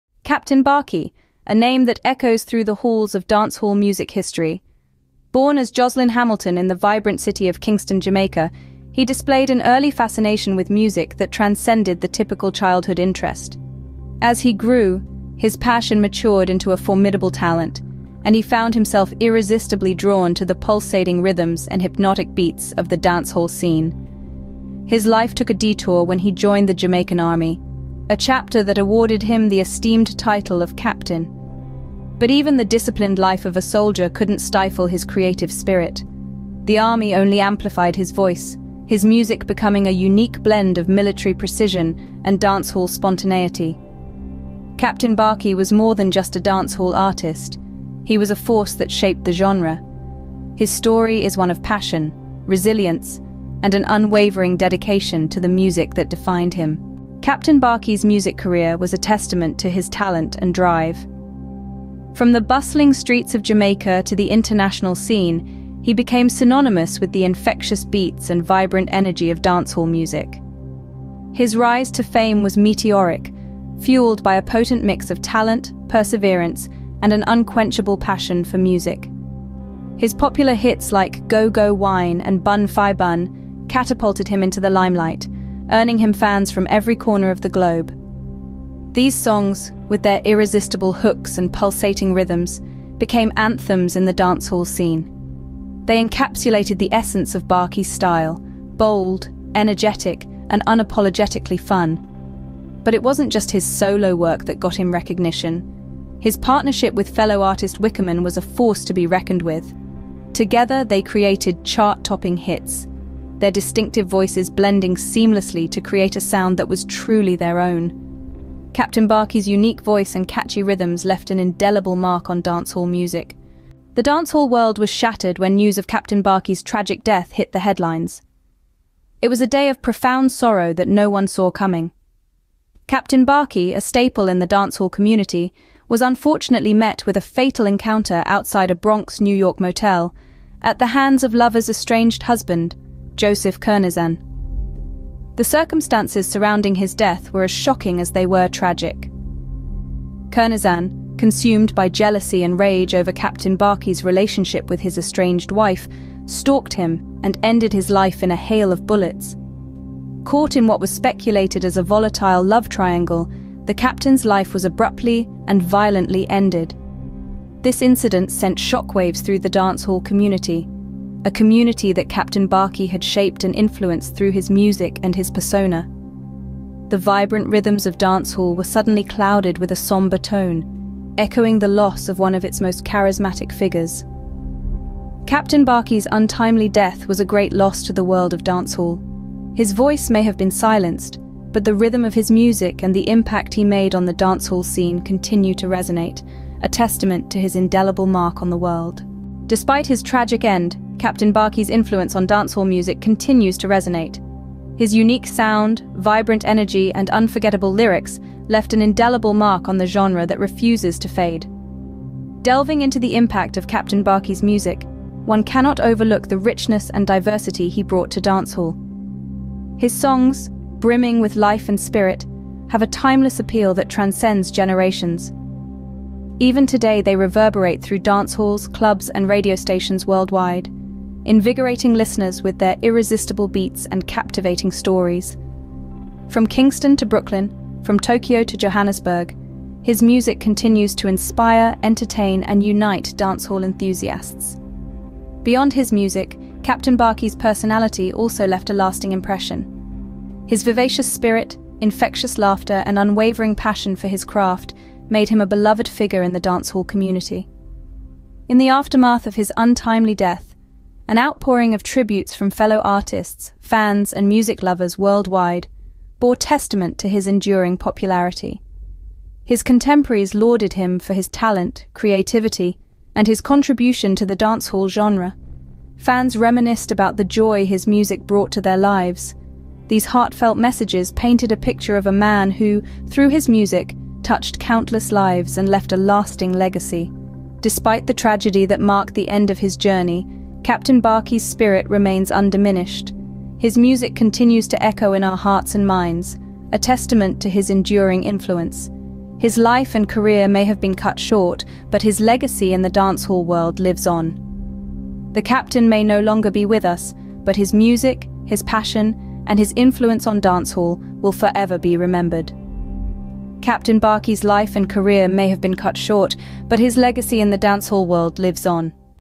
Jamaican Entertainer killed in the Bronx
Title: "Tragic News: Captain Barkey Fatally Shot in the Bronx"

Description: "In a devastating turn of events, Jamaican dancehall artist Captain Barkey was fatally shot in the Bronx. This shocking incident has left fans and the music community in mourning. Join us as we explore the details surrounding this tragic event and remember the legacy of Captain Barkey. Our thoughts go out to his loved ones during this difficult time."